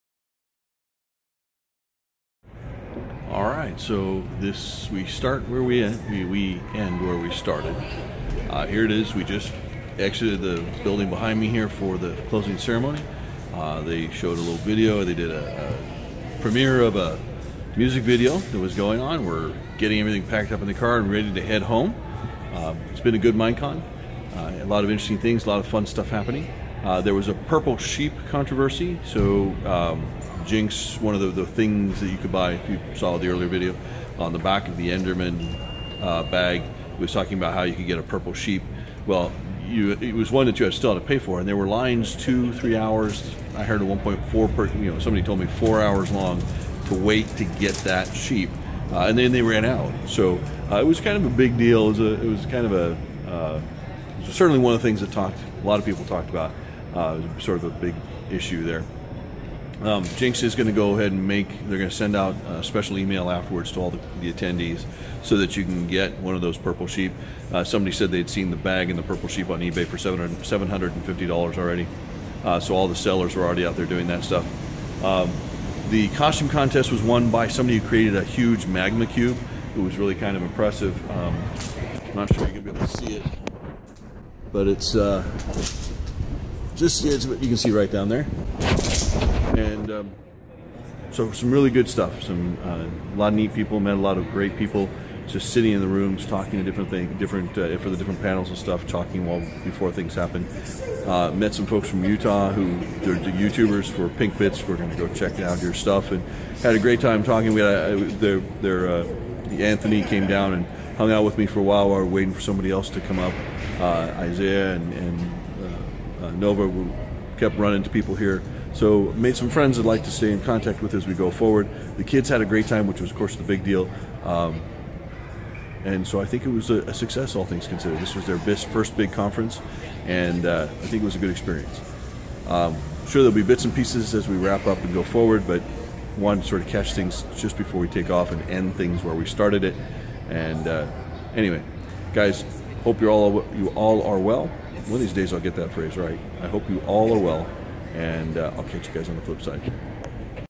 Minecon 2016 - We end where we began.
Minecon 2016 is over and I share some of our experiences.